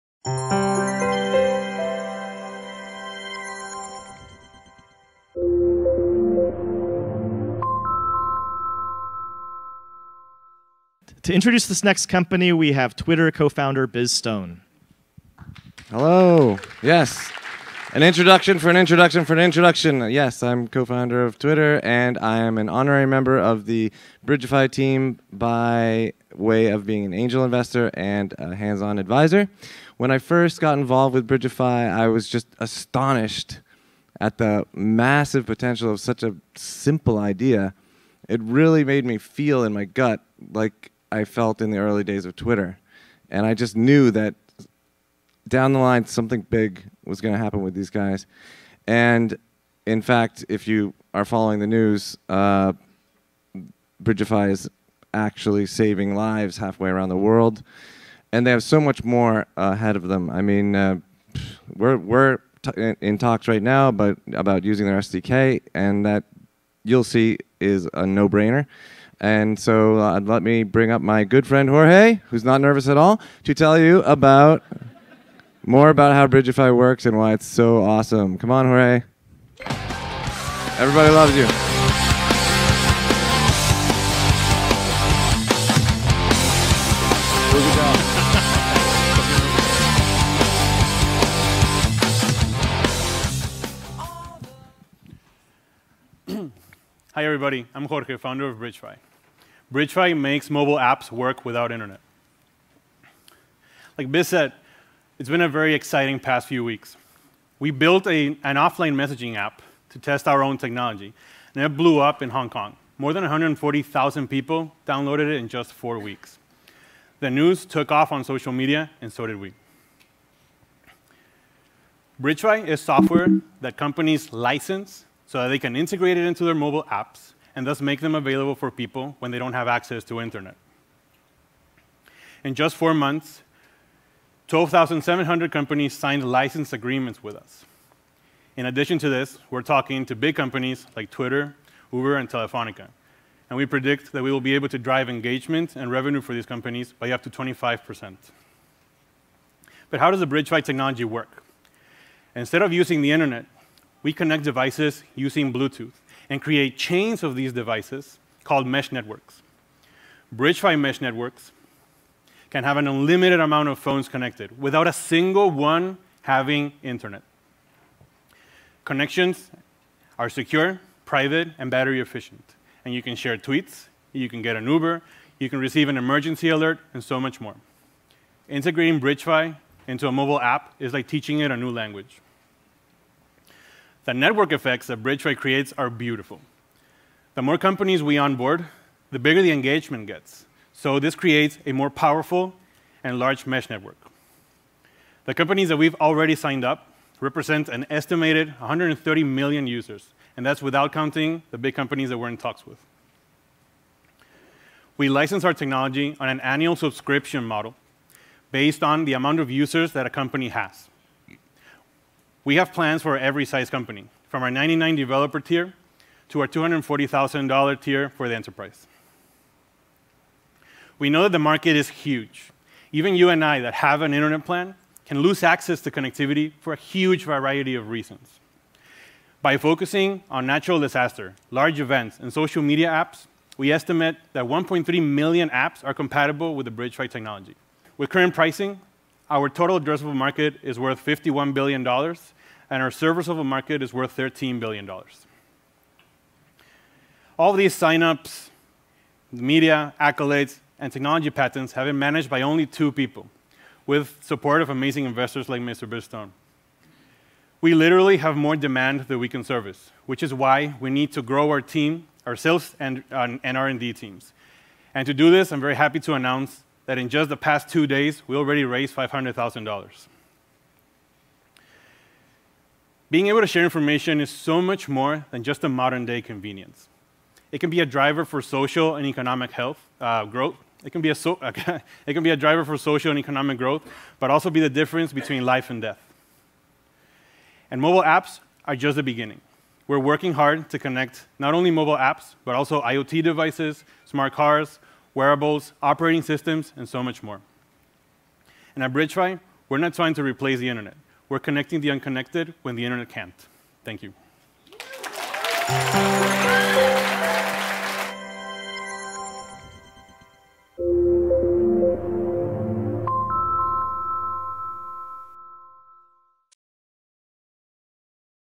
Bridgefy – Alchemist Accelerator Demo Day Pitch
Bridgefy pitched at the Alchemist Accelerator Demo Day in September 2019. Special intro by Twitter founder Biz Stone. to learn more about our mobile messaging app and our SDK.